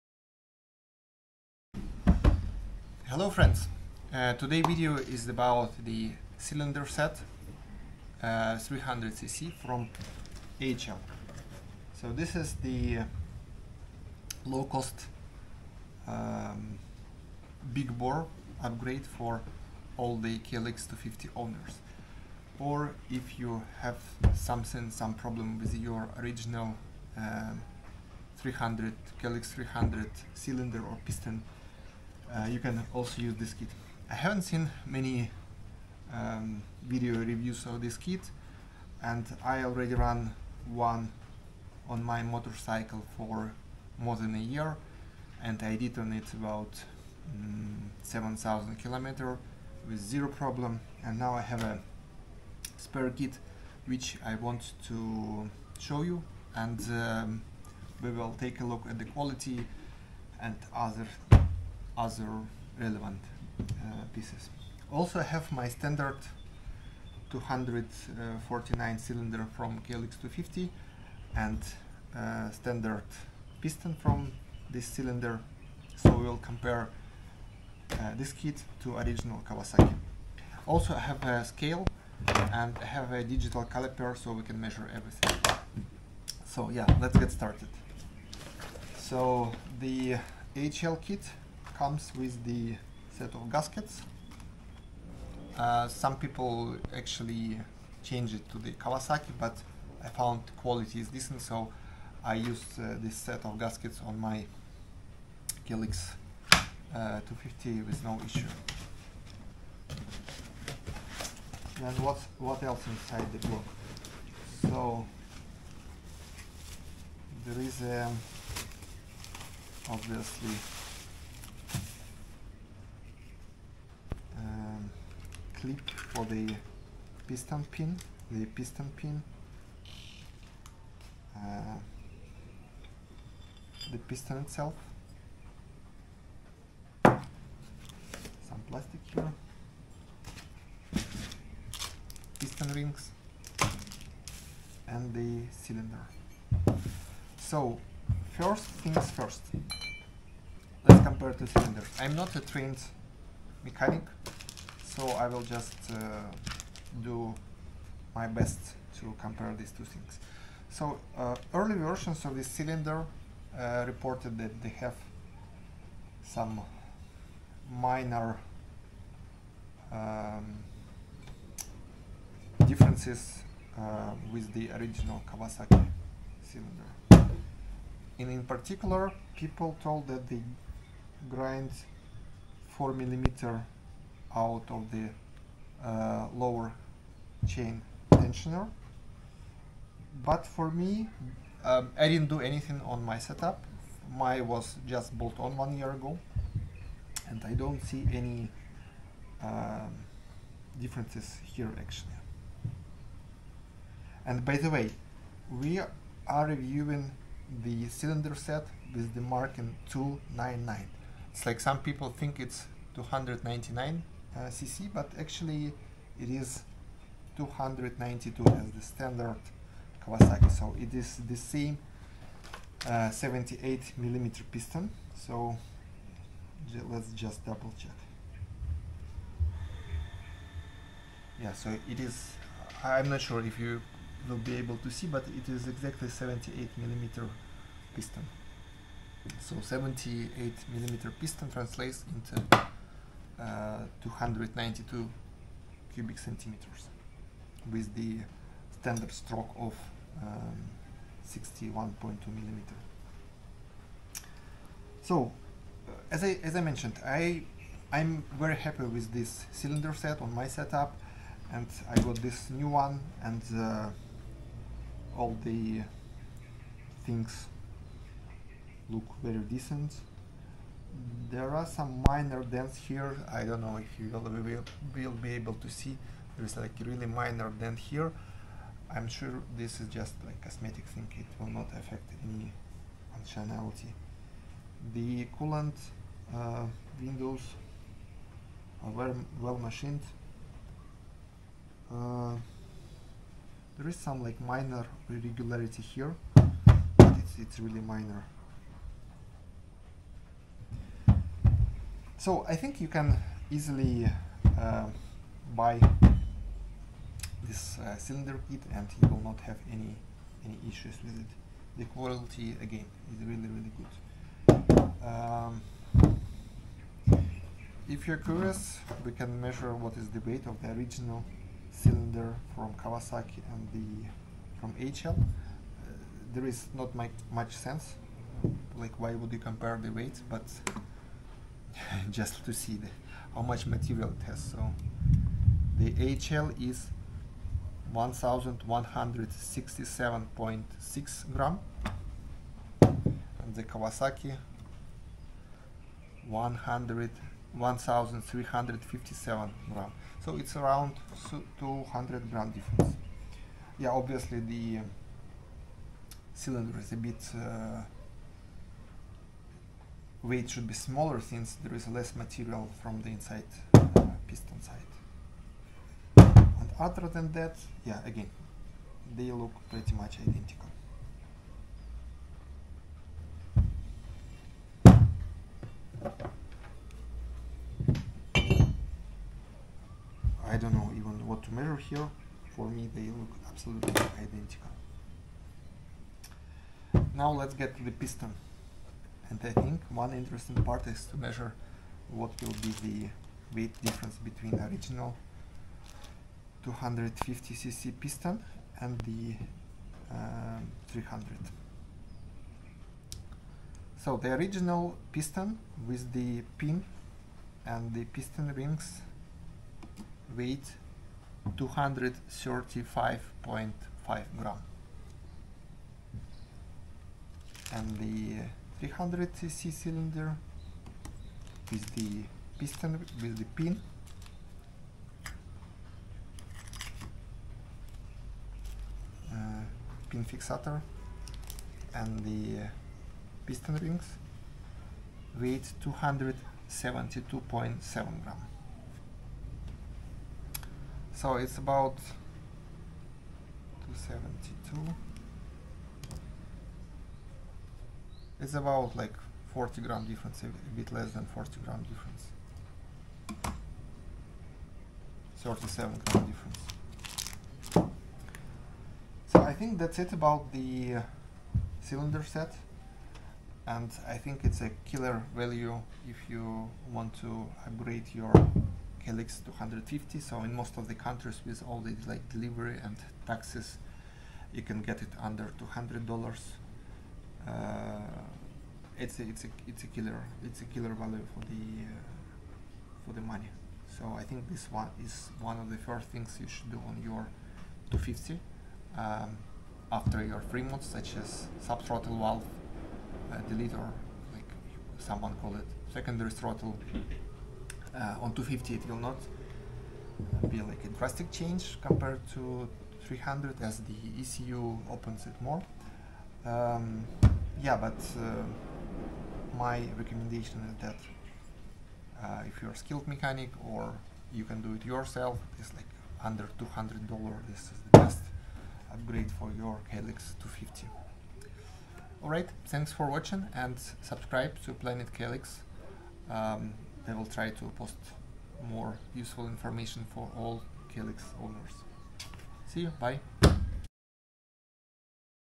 Review of the 292cc AHL "299" cylinder kit for Kawasaki KLX250/KLX300
Piston weight:
OEM 72mm piston (249cc cylinder) is 235.5g (including pin and piston rings)
AHL 78mm piston (292cc cylinder) is 272.7g (including pin and piston rings)

Timeline:
00:01 Intro
01:03 Standard OEM Kawasaki 249cc cylinder and piston
01:30 Unboxing AHL kit
02:30 Comparing OEM 249 cylinder to AHL cylinder
03:25 What "299" mark mean
03:40 Measuring diameter of the AHL piston - 78mm
04:02 "299" kit is a 292cc kit
04:15 A more detailed look at the AHL cylinder
05:18 Cylinder weight comparison
06:25 Pistons weight comparison
07:43 AHL kit is a great value for the money